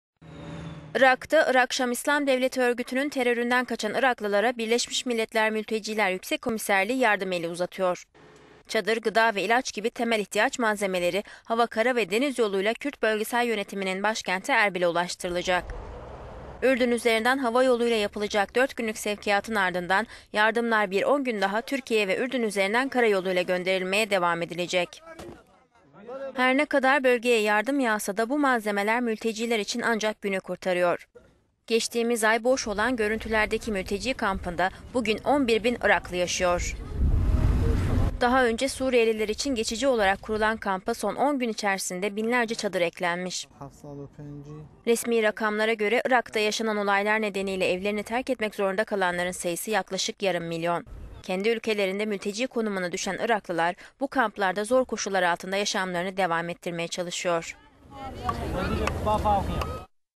BM Iraklı mültecilere yardım ulaştırıyor
Irak'ta, Irak Şam İslam Devleti (IŞİD) örgütünün teröründen kaçan Iraklılara Birleşmiş Milletler Mülteciler Yüksek Komiserliği yardım eli uzatıyor.

Çadır, gıda ve ilaç gibi temel ihtiyaç malzemeleri hava, kara ve deniz yoluyla Kürt Bölgesel Yönetimi'nin başkenti Erbil'e ulaştırılacak.

Ürdün üzerinden hava yoluyla yapılacak 4 günlük sevkiyatın ardından yardımlar bir 10 gün daha Türkiye ve Ürdün üzerinden kara yoluyla gönderilmeye devam edilecek.

Her ne kadar bölgeye yardım yağsa dahi bu malze…